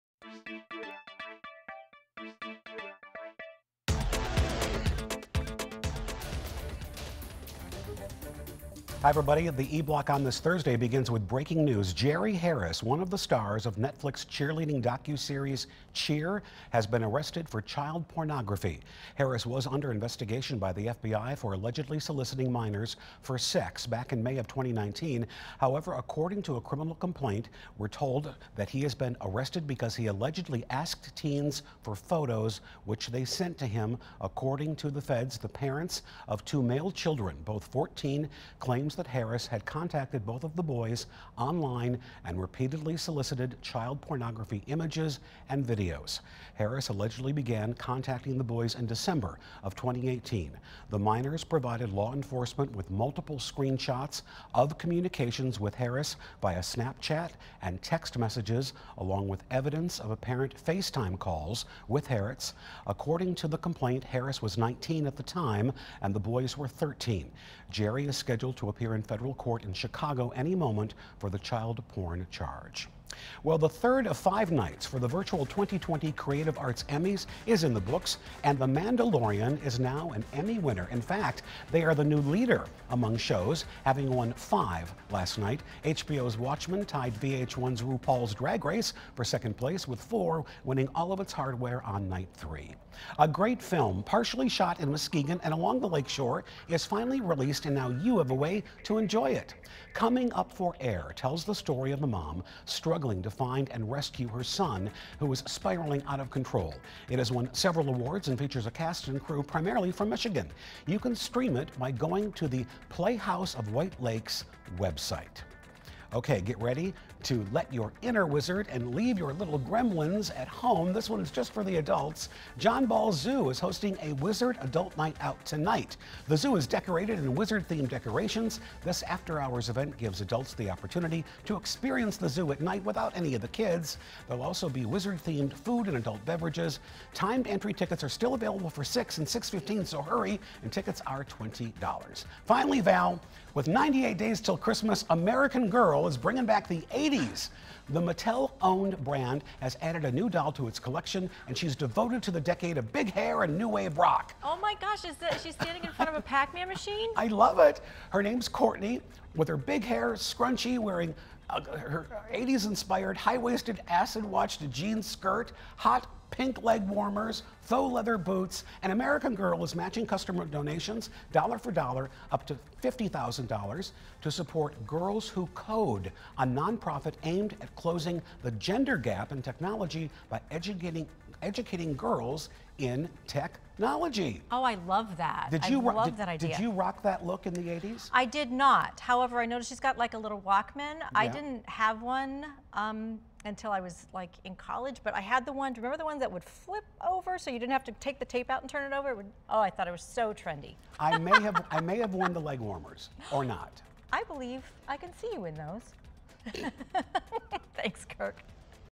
The Original E-Block with Kirk Montgomery
Here are some entertainment headlines from Thursday, Sept. 17.